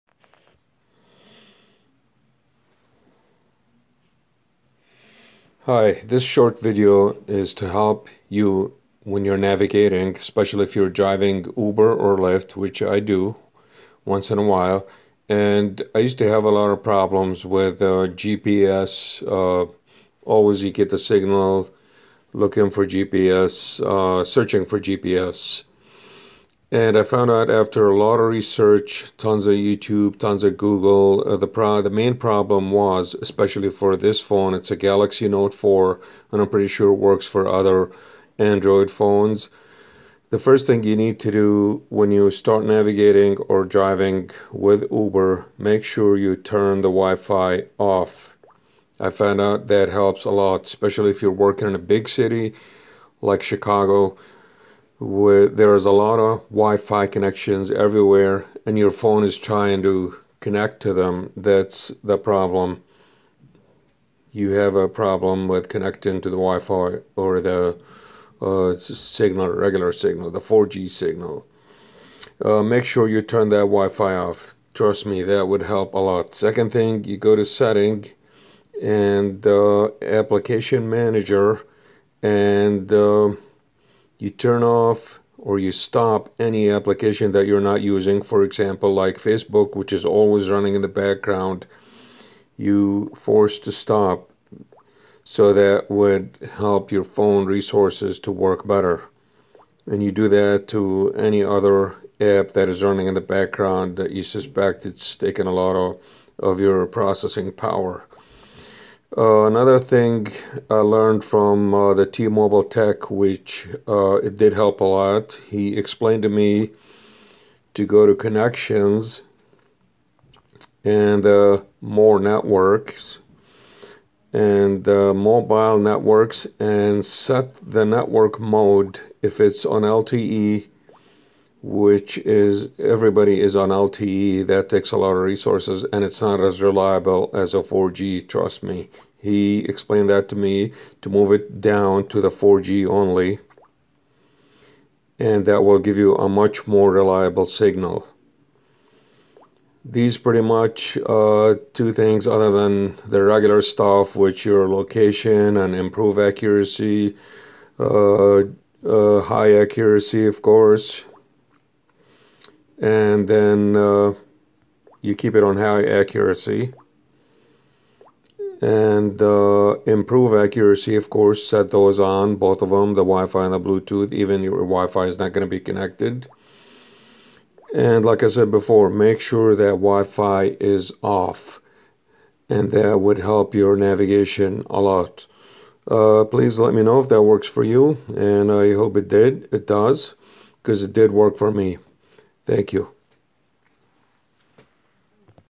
How to fix Uber GPS problems
GPS problems is a nightmare for Uber drivers or any body using their phone for Navigation, here are some quick tips that i learned over the years that helped a lot, Number 1 issue is the Wifi trust me on this one when you're using gps turn Wifi OFF, that will smooth your navigation, also networks mode move it down from LTE to 4G also helps a lot last thing especially with older phones turn off any app that is running in the background like Facebook and such, this will dedicate more of you processing power to Uber and GPS of course basic setting like location and high accuracy and off line maps , the Number 1 thing that made a huge difference for me is turning off Wifi wile navigating, thanks for watching i hope it helps